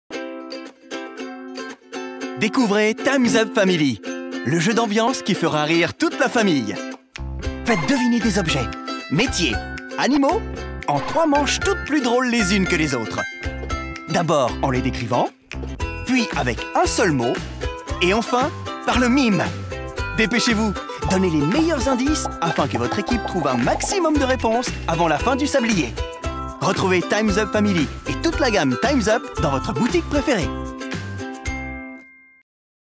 Time's Up! Family
Avec Time's Up! Family, amusez-vous à découvrir en famille ou entre copains des objets, des métiers et des animaux. Une partie de Time's Up! Family comporte trois manches. A tour de rôle, chaque joueur va disposer de 40 secondes pour faire deviner le plus de mots possible à ses coéquipiers. Animez vos soirées et jouez en famille avec ce jeu déjanté qui vous fera mourir de rire.

Time’s Up ! Family est un jeu d’ambiance pour jouer en famille ou entre copains. Les joueurs se divisent en équipe et se distribuent entre eux 30 cartes, plus deux cartes par joueur. Chaque joueur choisit alors 2 cartes qu’il juge trop difficiles et les remet dans la boîte. On mélange ensemble toutes les cartes restantes pour former la pioche, et la partie peut commencer. Une partie de Time’s Up ! Family comporte trois manches où les joueurs vont devoir faire deviner quels animaux, métiers ou objets sont inscrits sur les cartes de la pioche. Lors de chaque manche, à tour de rôle, chaque joueur va disposer de 40 secondes pour faire deviner le plus de mots possible à ses coéquipiers. Et chaque mot deviné rapportera 1 point à son équipe. Lors de la première manche, il pourra décrire les mots sans les nommer. Pour la deuxième manche, chaque joueur ne pourra utiliser qu’un seul mot. À la troisième et dernière manche, les joueurs qui font deviner n’ont plus le droit de parler ! Ils vont devoir mimer ou fredonner pour faire deviner les mots à leurs partenaires. À l’issue des trois manches, l’équipe qui a totalisé le plus de points remporte la partie.